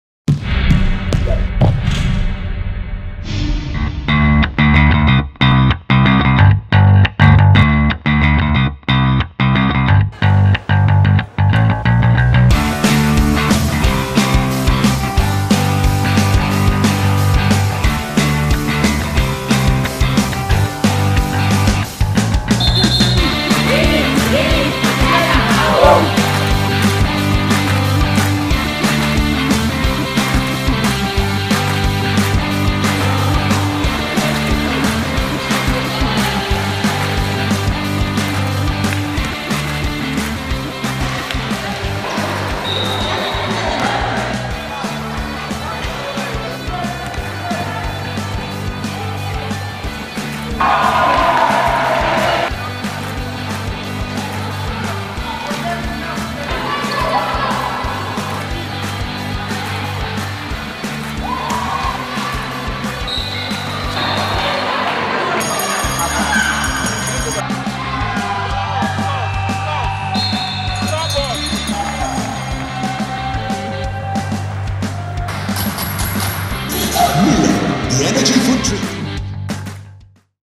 MBC 2016 - Confluence Conference (Girls Classification: Adamawa vs. Plateau)
Have a look at the highlights of the Classification game between Government Secondary School, Numan, Adamawa and Government High School, Gindiri, Plateau State.